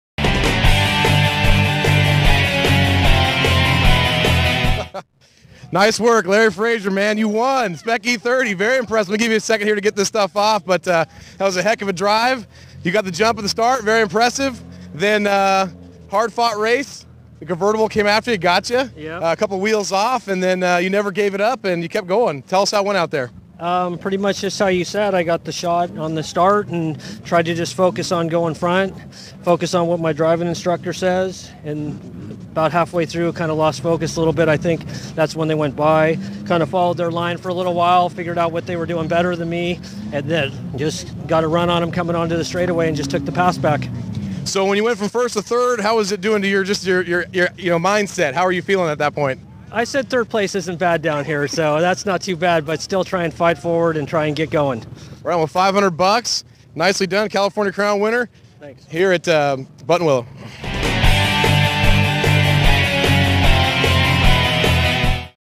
Larry Fraser 1st Pl at NASA California Crown 2012
Rob Krider speaks with Larry Fraser 1st place in Spec E30 at NASA's California Crown Weekend.